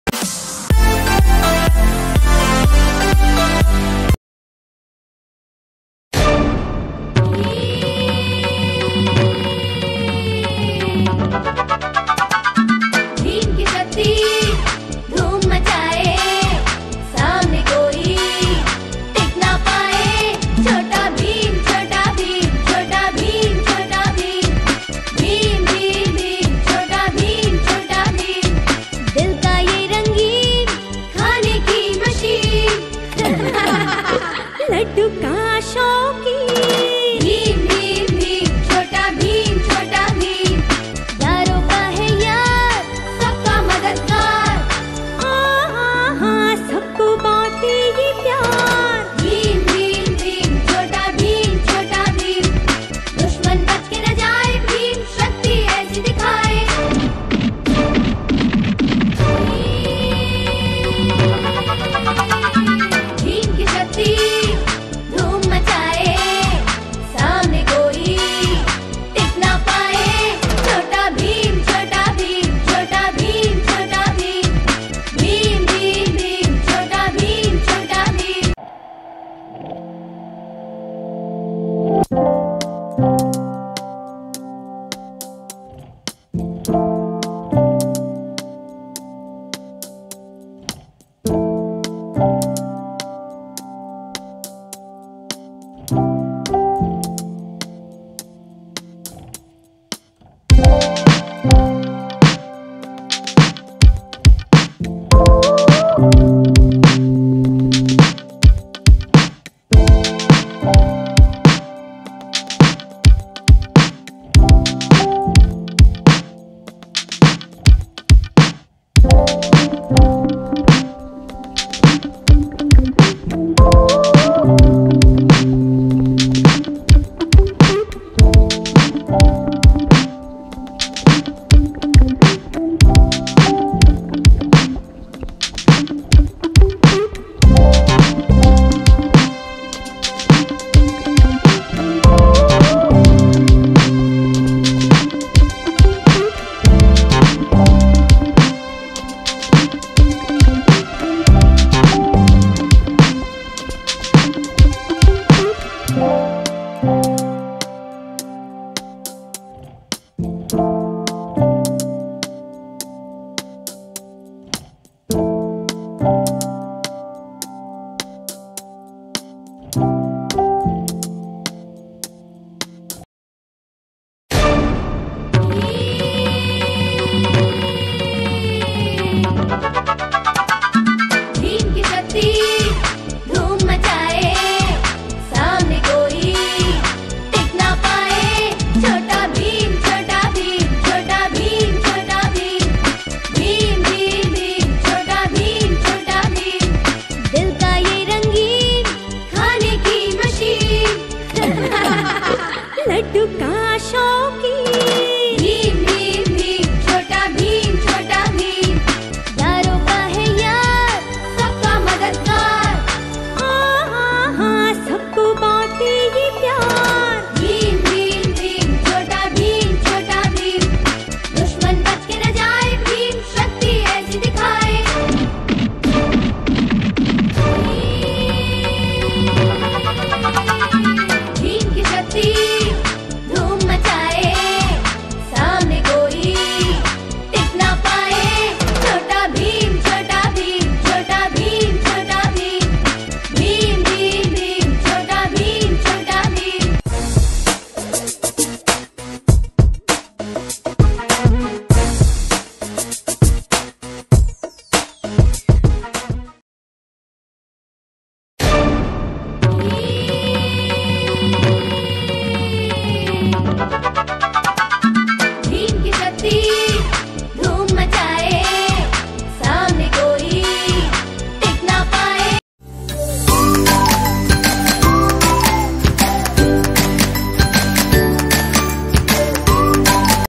kids painting Book || How to paint "kalia Pehlwan" Chota Bheem cartoon character
In this video we will learn how paint "kalia" cartoon characters.
Chota Bheem cartoon character 'kalia' painting.